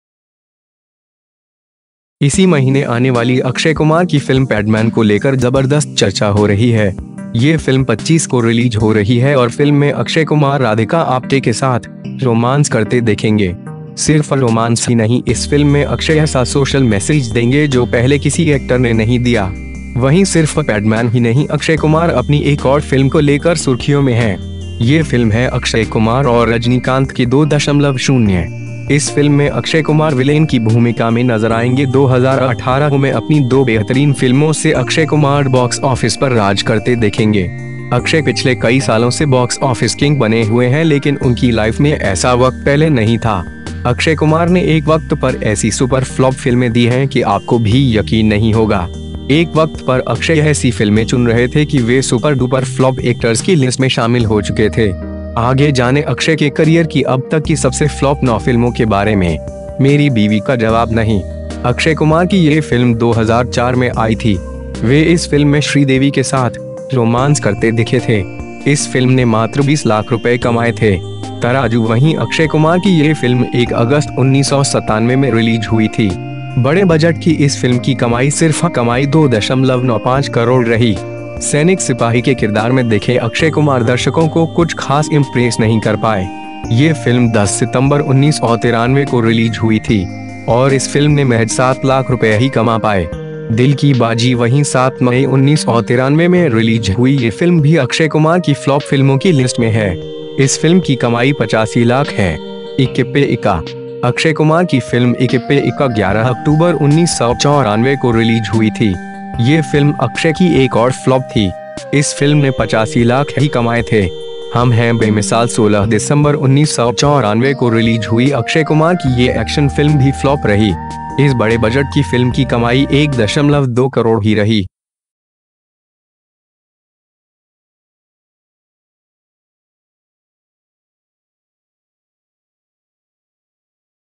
अक्षय कुमार सुपरहिट नहीं सुपरफ्लॉप पूरे 9 बार ये रहा
इसी महीने आने वाली अक्षय कुमार की फिल्म पैडमैन को लेकर जबरदस्त चर्चा हो रही है। ये फिल्म 25 को रिलीज हो रही है और फिल्म में अक्षय कुमार राधिका आप्टे के साथ रोमांस करते दिखेंगे। सिर्फ रोमांस ही नहीं इस फिल्म में अक्षय ऐसा सोशल मैसेज देंगे जो पहले किसी एक्टर ने नहीं दिया। वहीं सिर्फ पैडमैन ही नहीं अक्षय कुमार अपनी एक और फिल्म को लेकर सुर्खियों में हैं।

ये फिल्म है अक्षय कुमार और रजनीकांत की 2.0.. इस फिल्म में अक्षय कुमार विलेन की भूमिका में नजर आएंगे। 2018 में अपनी 2 बेहतरीन फिल्मों से अक्षय कुमार बॉक्स ऑफिस पर राज करते दिखेंगे। अक्षय पिछले कई सालों से बॉक्स ऑफिस किंग बने हुए हैं लेकिन उनकी लाइफ में ऐसा वक्त पहले नहीं था।

अक्षय कुमार ने एक वक्त पर ऐसी सुपर फ्लॉप फिल्में दी हैं कि आपको भी यकीन नहीं होगा। एक वक्त पर अक्षय ऐसी फिल्में चुन रहे थे कि वे सुपर-डुपर फ्लॉप एक्टर्स की लिस्ट में शामिल हो चुके थे। आगे जानें अक्षय के करियर की अब तक की सबसे फ्लॉप 9 फिल्मों के बारे में-

मेरी बीवी का जवाब नहीं
 अक्षय कुमार की ये फिल्म 2004 में आई थी। वे इस फिल्म में श्रीदेवी के साथ रोमांस करते दिखे थे। इस फिल्म ने मात्र 20 लाख रुपए कमाए थे।

तराजू
वहीं अक्षय कुमार की ये फिल्म 1 अगस्त 1997 में रिलीज हुई थी। बड़े बजट की इस फिल्म की कमाई सिर्फ कमाई 2.95 करोड़ रही।

सैनिक
सिपाही के किरदार में दिखे अक्षय कुमार दर्शकों को कुछ खास इंप्रेस नहीं कर पाए। ये फिल्म 10 सितंबर 1993 को रिलीज हुई थी। और इस फिल्म ने महज 7 लाख रुपए ही कमा पाए।

दिल की बाजी
वहीं 7 मई 1993 में रिलीज हुई ये फिल्म भी अक्षय कुमार की फ्लॉप फिल्मों की लिस्ट में है। इस फिल्म की कमाई 85 लाख है।

इक्के पे इक्का
 अक्षय कुमार की फिल्म इक्के पे इक्का 11 अक्टूबर 1994 को रिलीज हुई थी। ये फिल्म अक्षय की एक और फ्लॉप थी। इस फिल्म ने 85 लाख ही कमाए थे।

हम हैं बेमिसाल
16 दिसंबर 1994 को रिलीज हुई अक्षय कुमार की ये एक्शन फिल्म भी फ्लॉप रही। इस बड़े बजट की फिल्म की कमाई 1.2 करोड़ ही रही